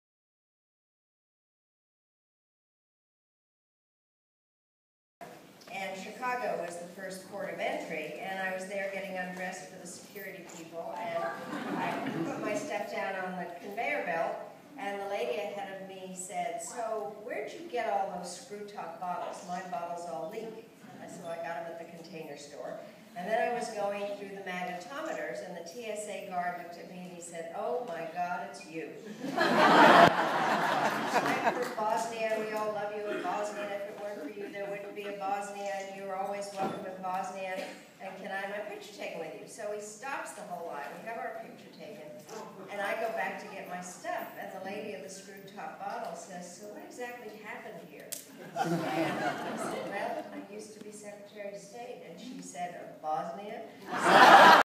Dr Madeline Albright
A moment of humor with Dr. Madeline Albright.  Dr. Albright tells a short story about  how some people recognize her in her travels as former Secretary of State for the Clinton Administration.